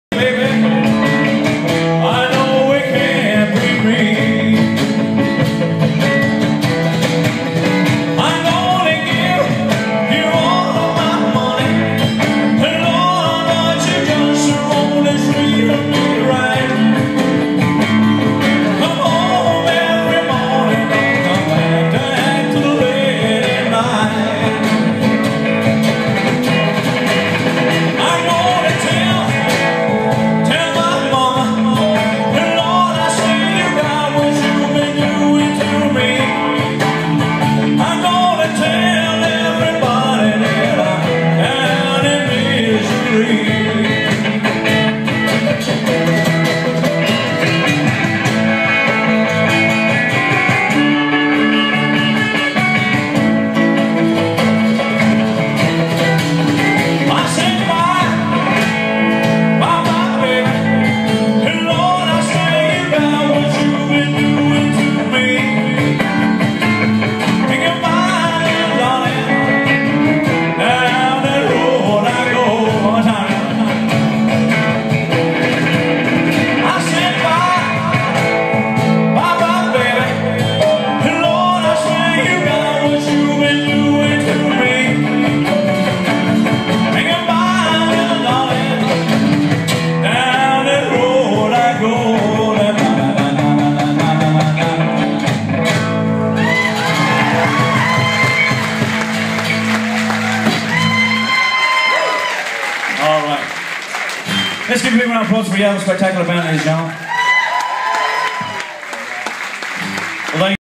CC HOULIHAN Elvis TCB BAND AND SWEET SENSATIONS
ARMAGH MARKET PLACE FRIDAY 25th OCT 2013... Start of the night some 68 style